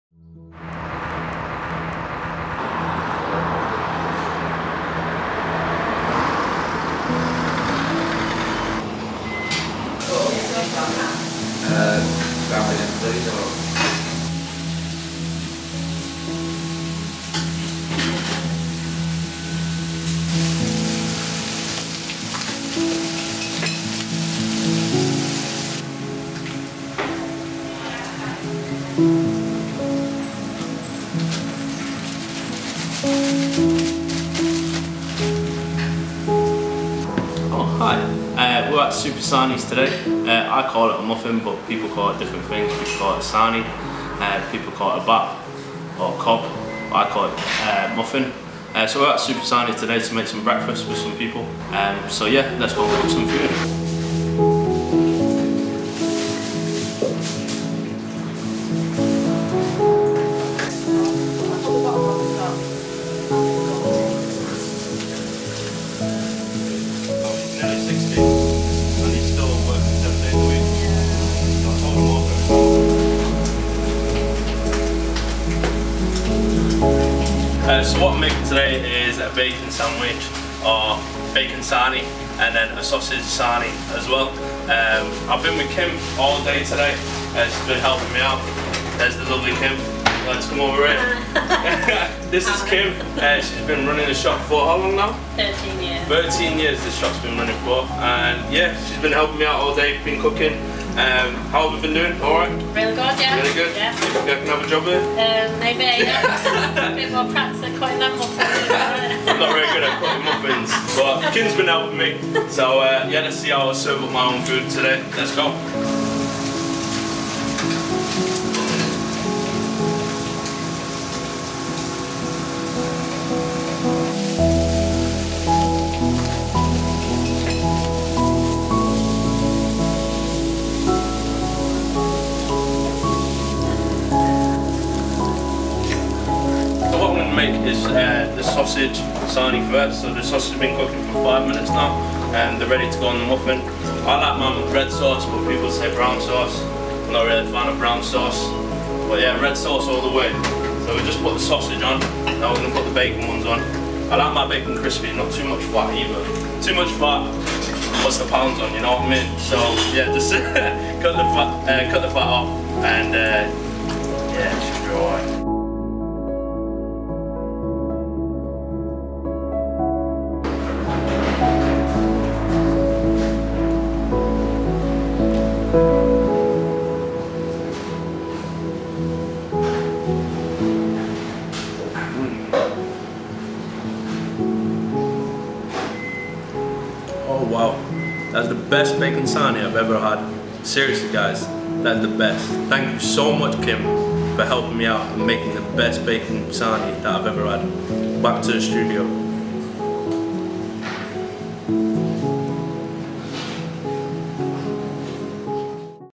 Supa Sarnies
Here is a video tape that I have created for my cooking show. I present  the video  to give the audiences some information about the cafe' and why I was there, I hope you enjoy the video, and make sure to look out for more videos in 2019.